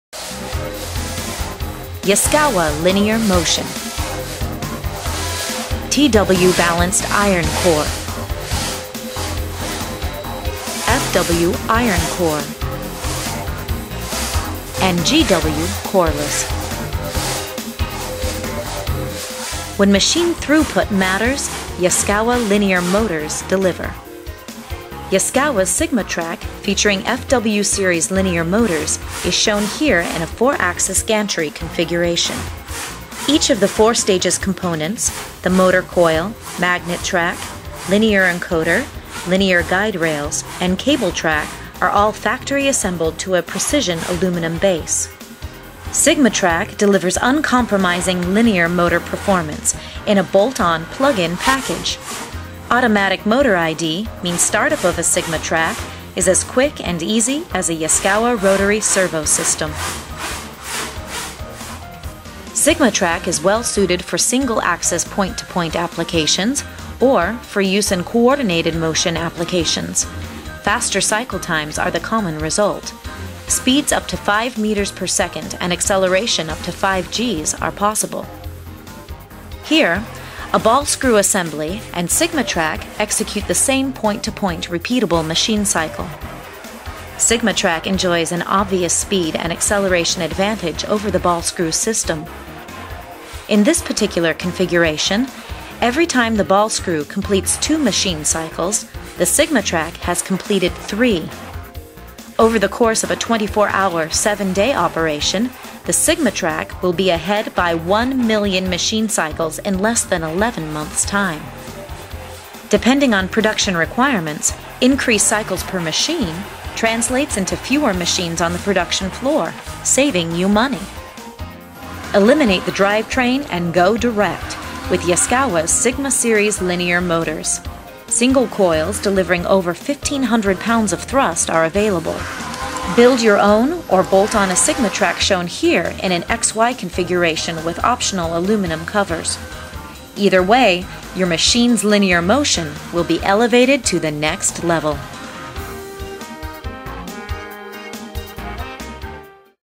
Yaskawa Linear Motors - Sigma Trac beat Ball Screw
Sigma Trac beats ball screw by 1 million machine cycles over a year's time. Yaskwawa's Sigma series of linear motion and motor products is highlighted in this video from Yaskawa Motion.